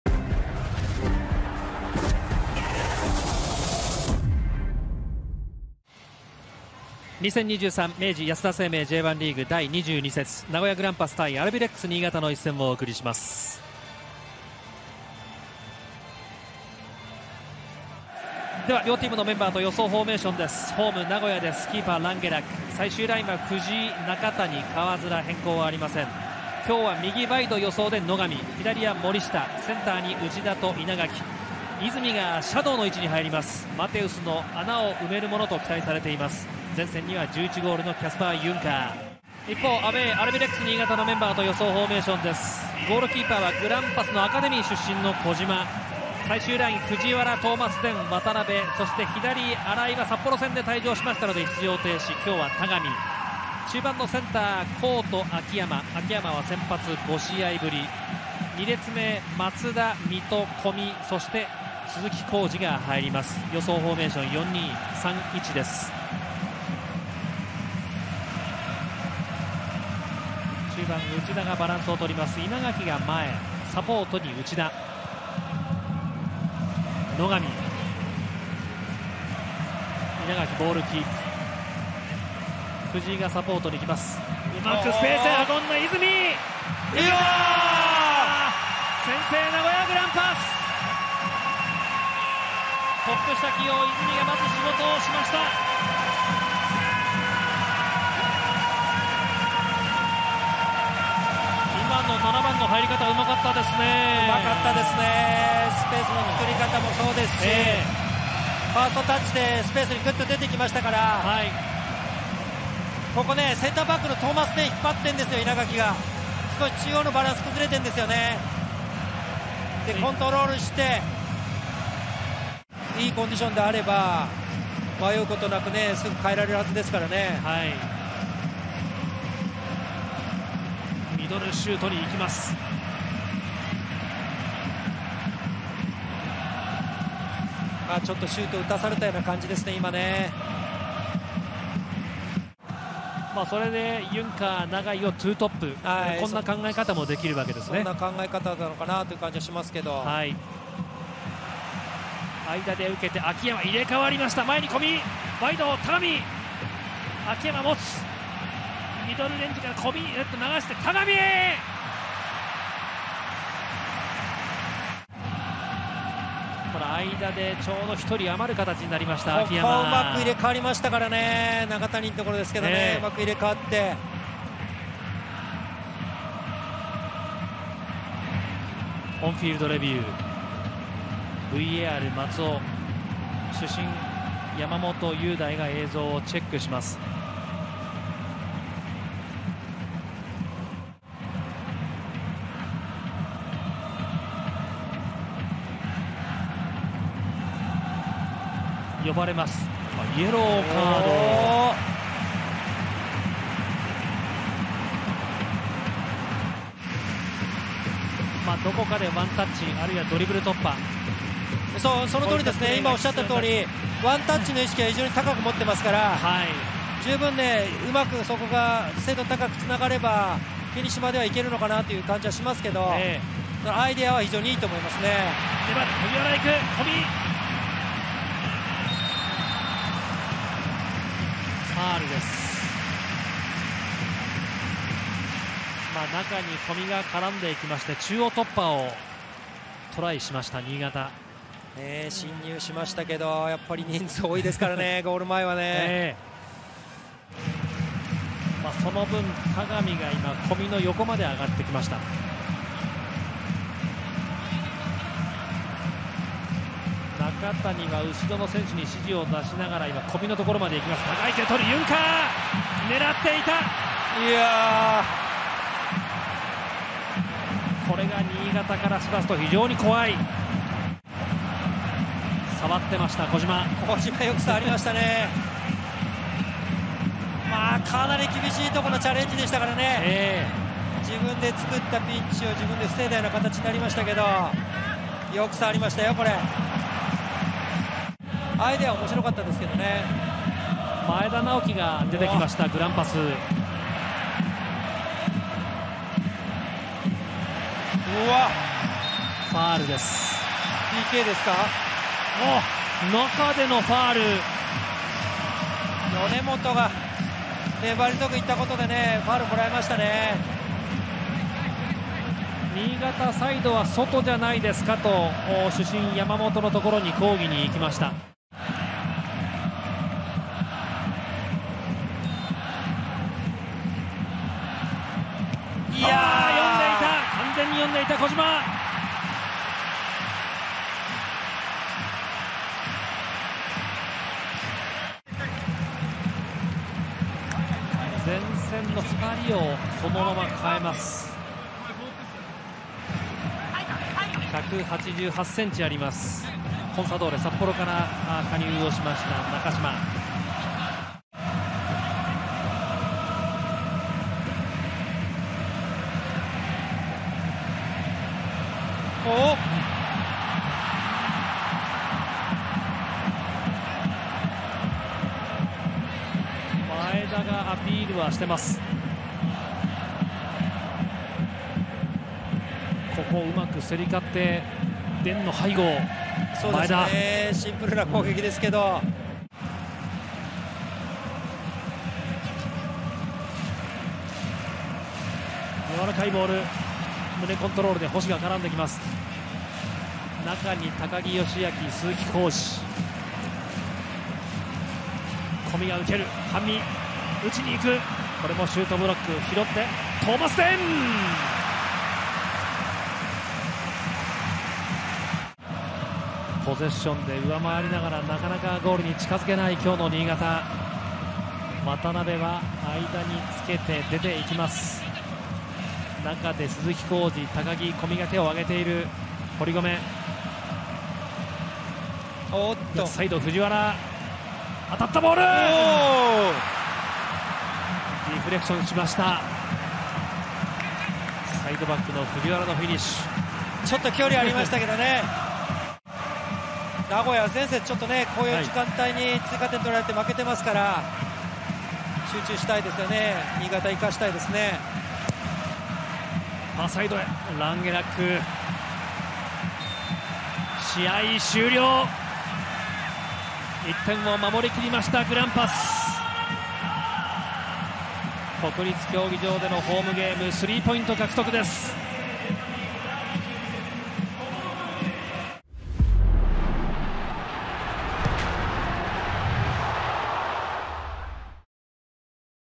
【DAZNハイライト】名古屋グランパス vs アルビレックス新潟 (H) ２０２３明治安田生命Ｊ１リーグ 第22節
◇2023年8月5日（土）19:00 KICK OFF「名古屋グランパス vs アルビレックス新潟」@国立競技場